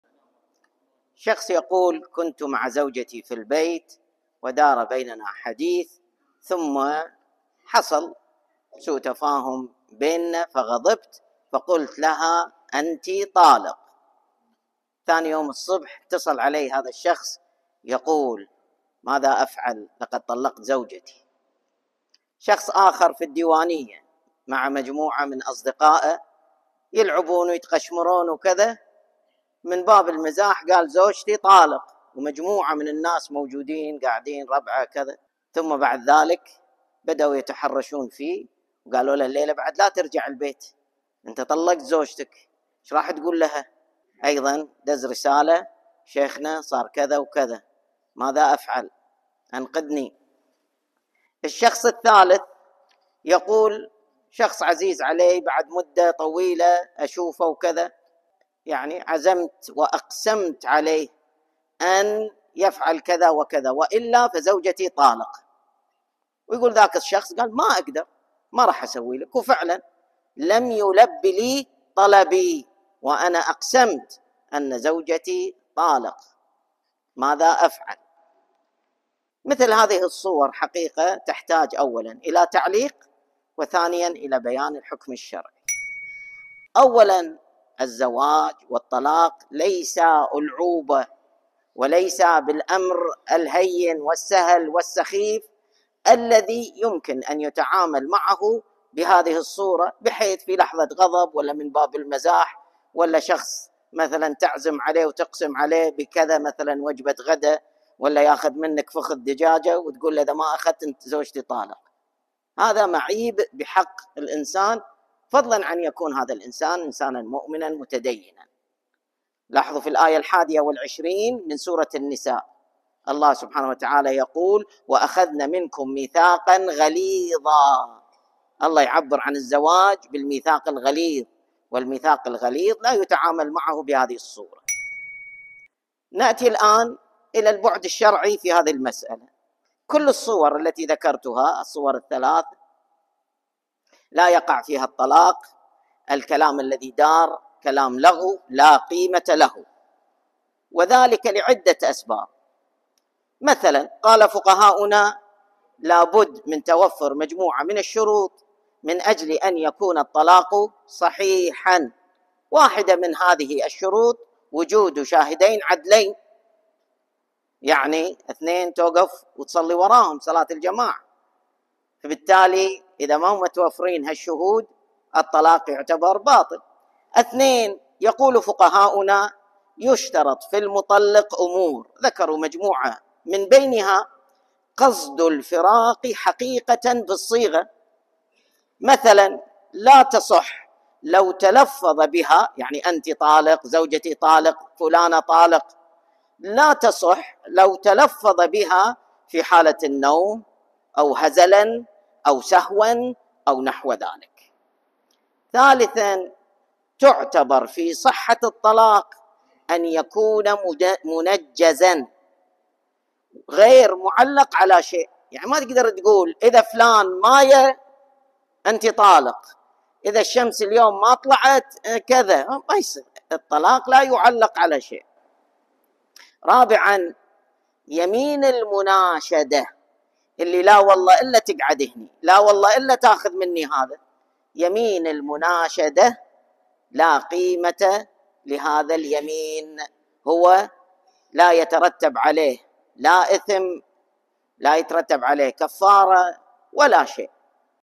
هل أصبحت زوجتي طالق ؟
- غضبت على زوجتي، وقلت لها: أنتِ طالق... فهل وقع الطلاق؟
- كنت في الديوانية وقلت من باب المزاح: زوجتي طالق... فهل وقع الطلاق؟
- عزمت وأقسمت على إنسان عزيز علي أن يفعل شيئاً وإلا فزوجتي طالق، ولم يفعل... فهل وقع الطلاق؟
1. الزواج والطلاق ليسا ألعوبة ولا أمراً سخيفاً كي يتعامل معهما بهذه الصورة. في الآية 21 من سورة النساء، تم التعبير عن علاقة الزواج بالميثاق الغليظ: (وَأَخَذْنَ مِنكُم مِّيثَاقًا غَلِيظًا).
2. لا يقع الطلاق في أي صورة من الصور السابقة، وكل ذلك يعتبر لغواً. للأسباب التالية:
- قال فقهاؤنا:
1) لابد من توفر عدة أمور ليكون الطلاق صحيحاً، ومن بينها وجود شاهدين عدلين عند الطلاق.
2) (يشترط في المطلِّق أمور) ثم ذكروا مجموعة منها، ومن بينها: (قصد الفراق حقيقة بالصيغة، فلا يصحّ الطلاق إذا صدرت الصيغة من السكران ونحوه ممّن ليس له قصد معتدّ به، كما لا تصحّ لو تلفّظ بها في حالة النوم أو هزلاً أو سهواً أو نحو ذلك).
3) (تعتبر في صحّة الطلاق أن يكون منجّزاً، غير معلّق على شيء).
4) يمين المناشدة لا يترتّب عليه شيء من إثم ولا كفّارة، لا على الحالف في إحلافه، ولا على المحلوف عليه في حنثه وعدم إنجاح مسؤوله.